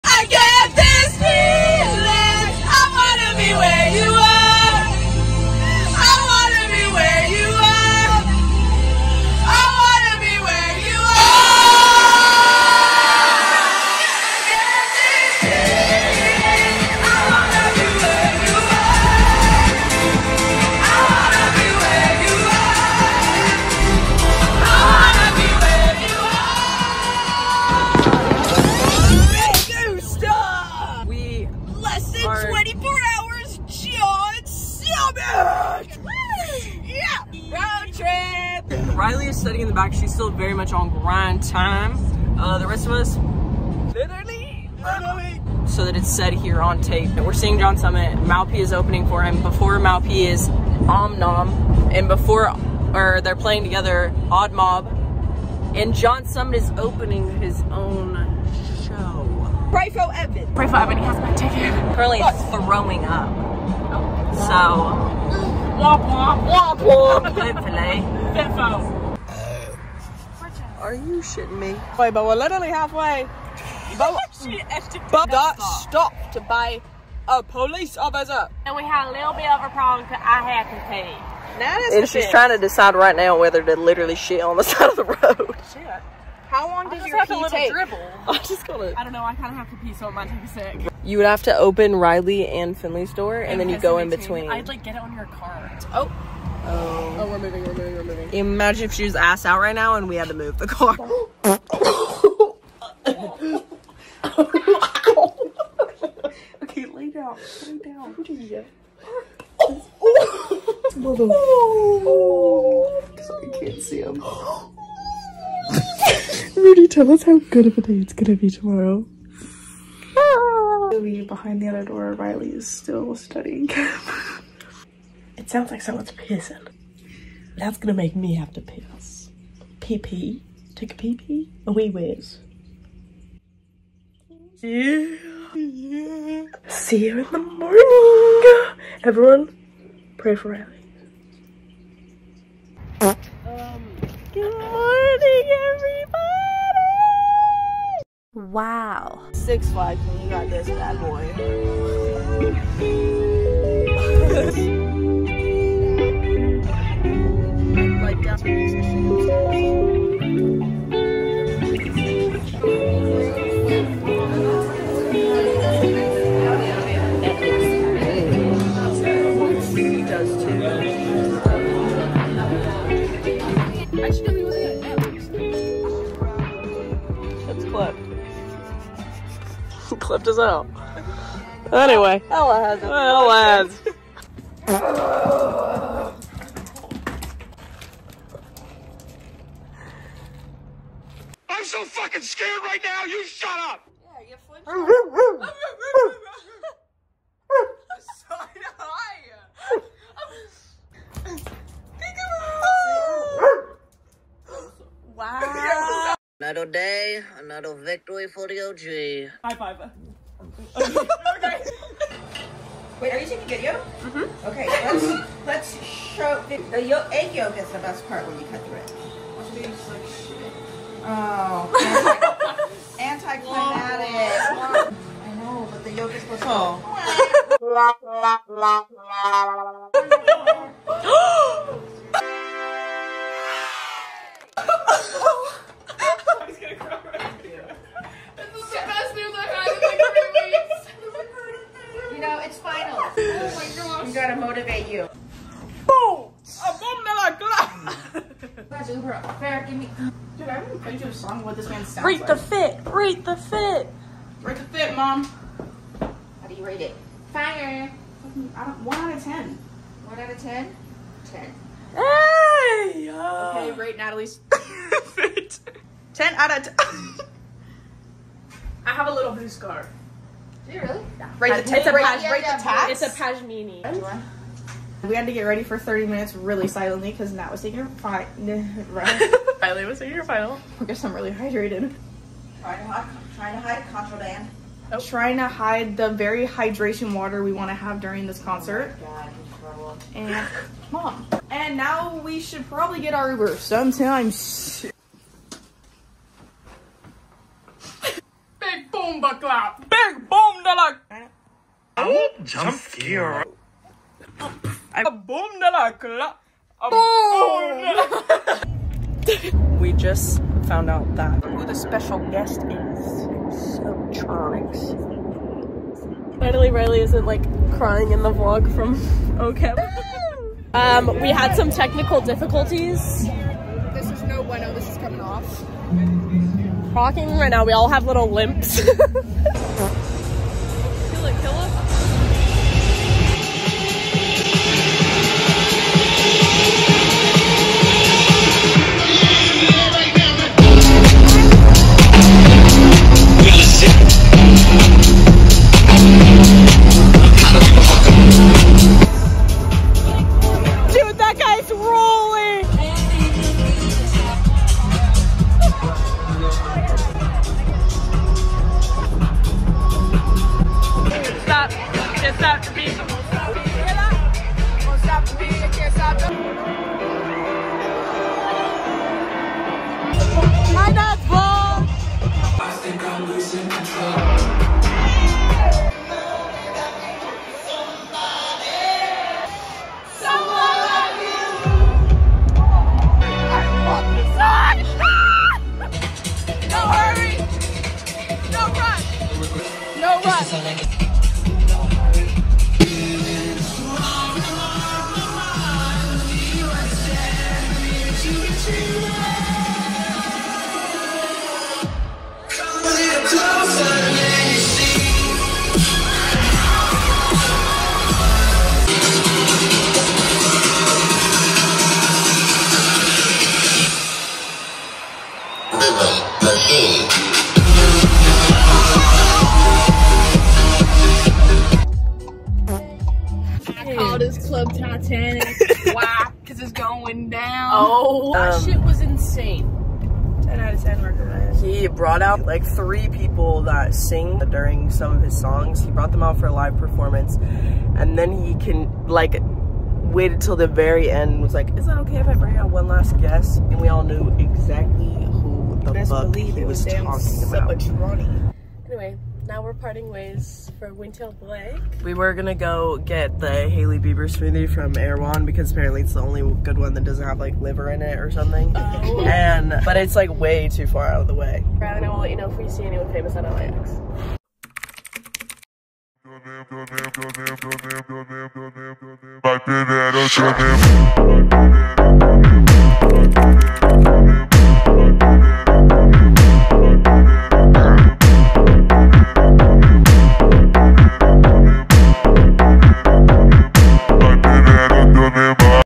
JOHN SUMMIT VLOG 5
Come with us to spend a night in Long Beach and see John Summit+openers at BMO.